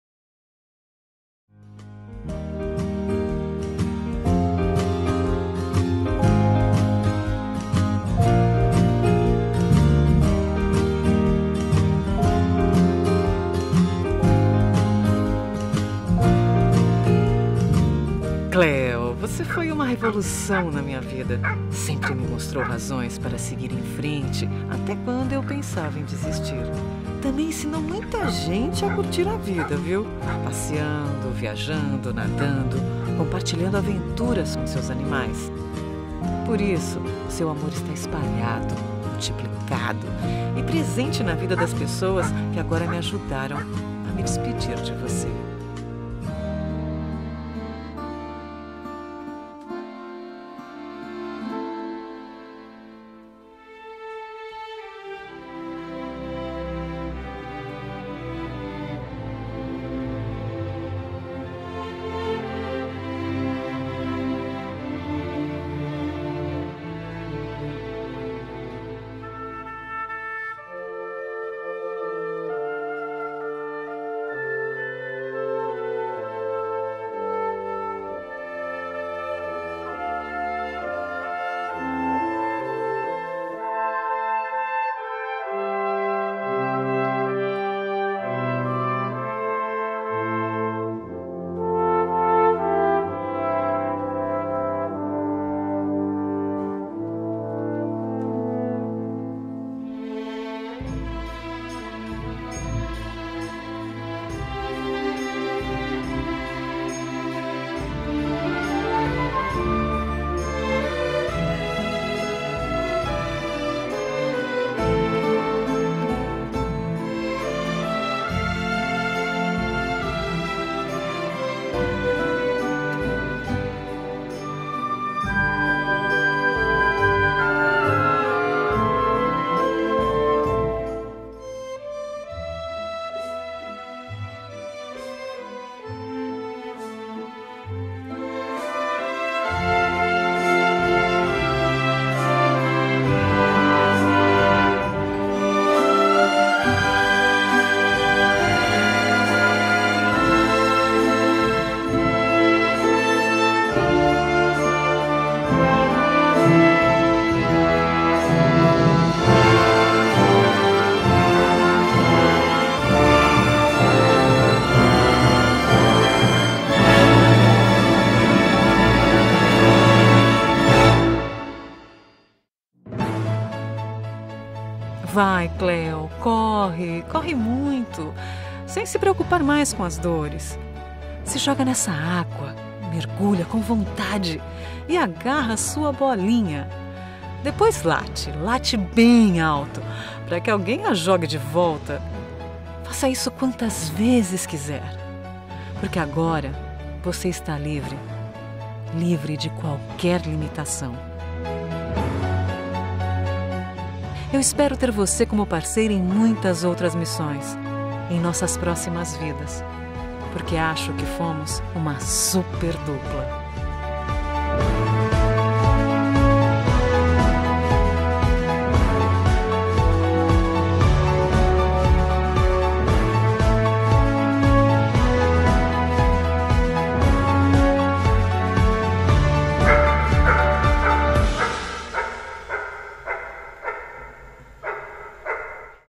Cleópatra Breezy | 02/01/2007 - 13/11/2016
Cerimônia de despedida da Cléo no Rio Juquiá, Juquitiba - SP, em 27/11/2016.

Turismo 4 Patas, evento Patas na Trilha 81 - Edição de Aniversário